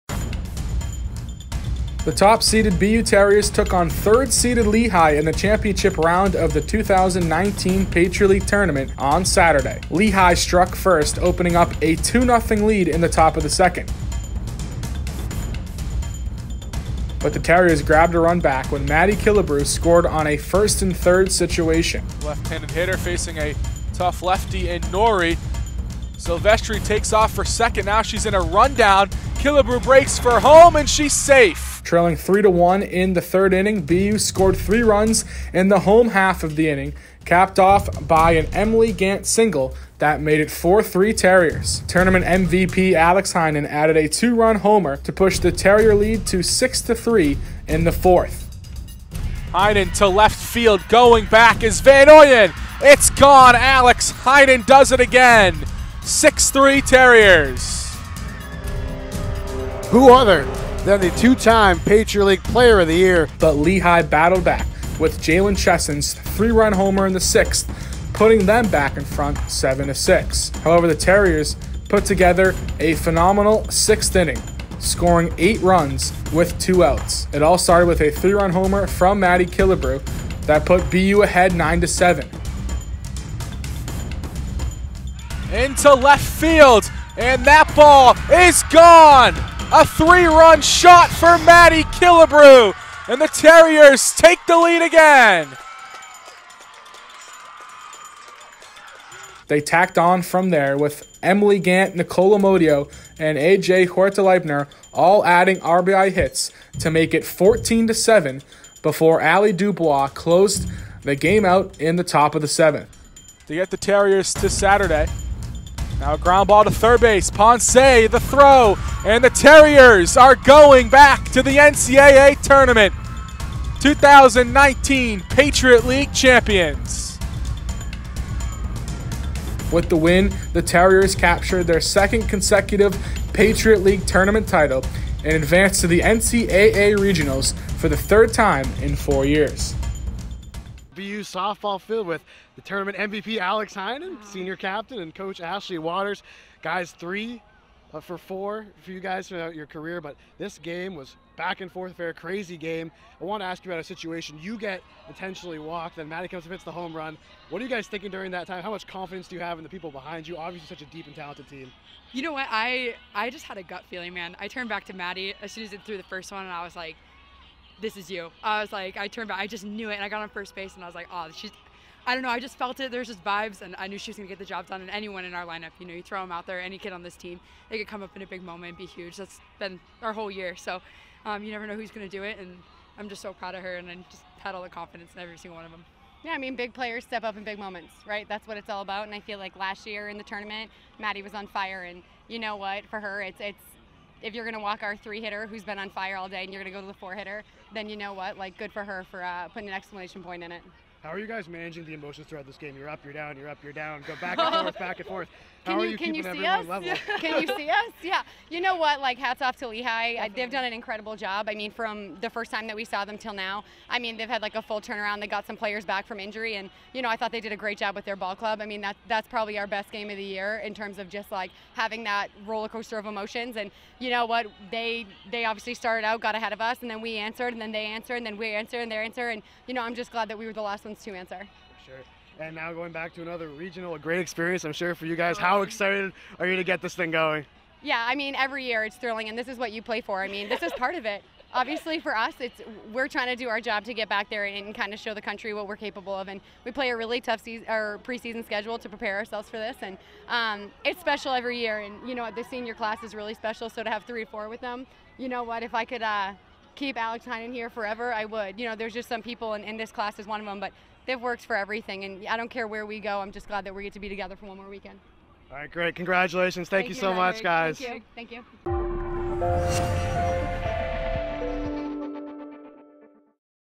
Patriot League Softball Championship Highlights: BU vs Lehigh (5/11/2019)
Boston University softball repeated as Patriot League champions with a thrilling 14-7 victory over Lehigh.